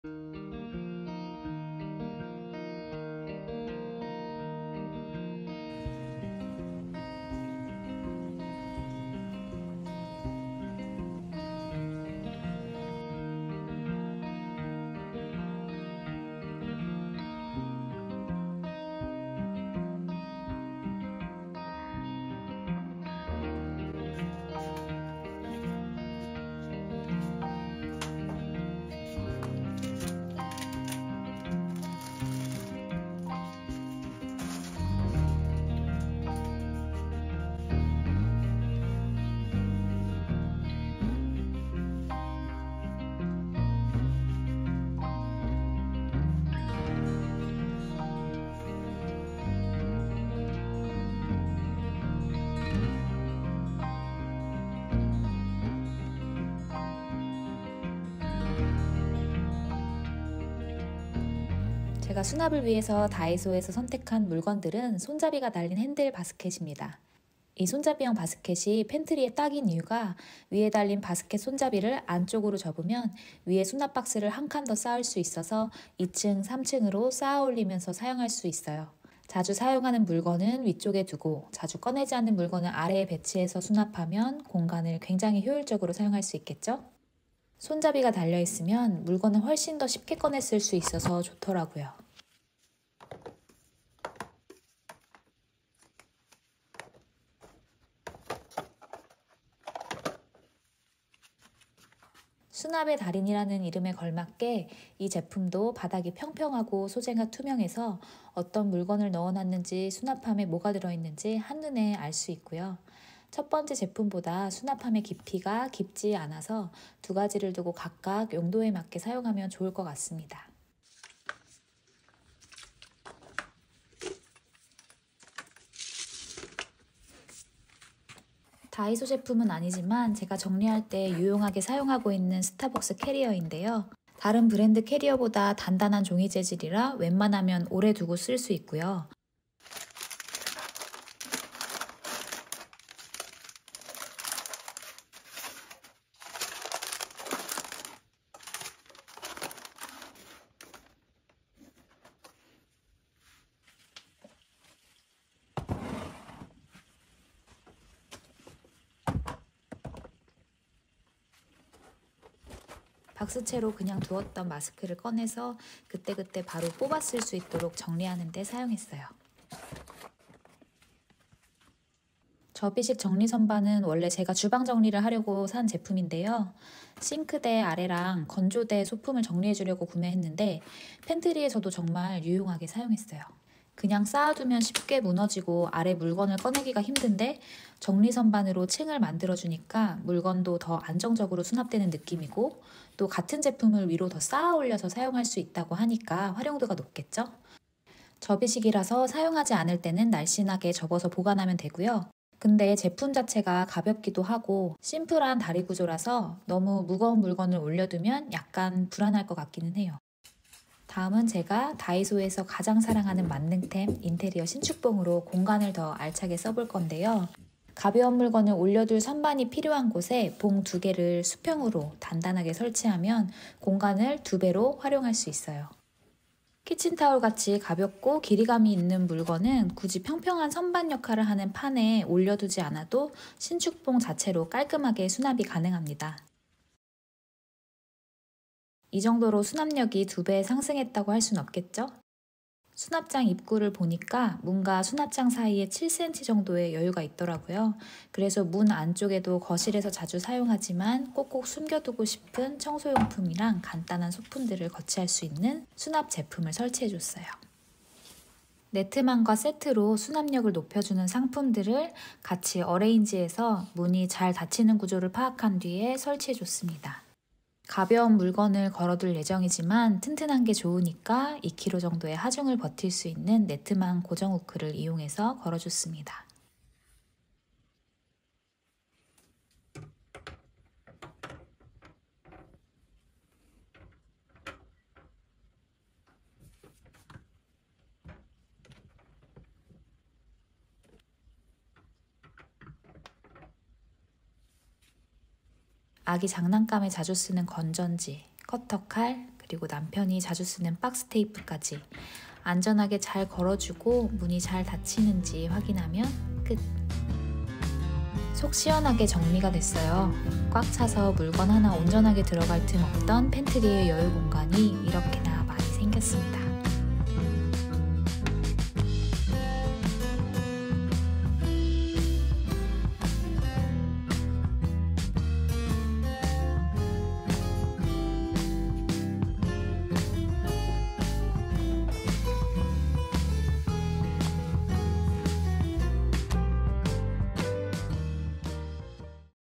절대 후회없는 다이소꿀템으로 | 수납장 정리 | 단정한 정리정돈팁 | 미니멀라이프 정리 수납하는 노하우
다이소에서 수납 능력을 2배 높여주는 아이템으로
작은 수납장 팬트리를 정리했어요.
뒤죽박죽한 물건들을 싹 거둬내고 물건을 먼저 버리고 분류하고 잘 보이게 정리하면, 시간이 지나 못쓰는 물건 없이 단정한 상태를 유지할 수 있습니다.

빽빽했던 공간이 속시원하게 넓어지니 수납 정리 방법에 대해
더 고민하고 연구하게 되네요.
정리가 주는 힘! 소소하지만 작은 팁들도 준비했어요.

☝🏻다이소에서 구입한 제품들은 상품명과 품번을 기입해두었습니다.♥ ັ
영상 속의 정보를 확인해주세요!